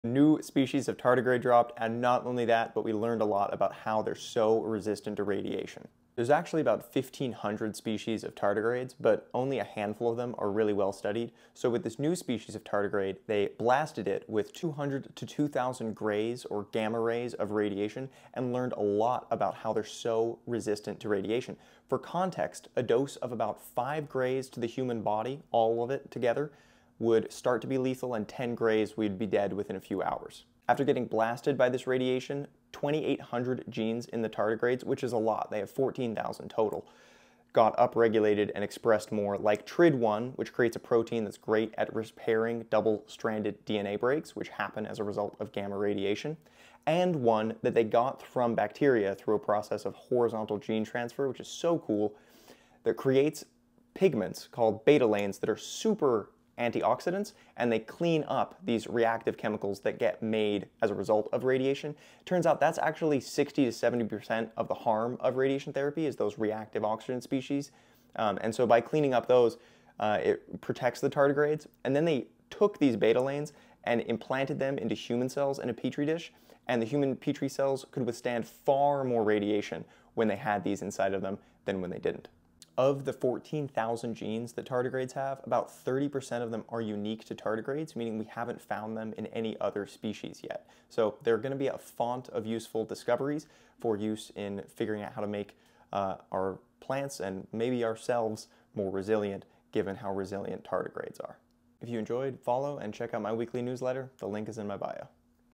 New Tardigrade Radiation Resistance Mechanisms
There’s more biology knowledge out there already than any single human can study and process, and so much more to discover.
Sources:
1) Multi-omics landscape and molecular basis of radiation tolerance in a tardigrade (Science)
2) New species of tardigrade reveals secrets of radiation-resisting power (Nature News)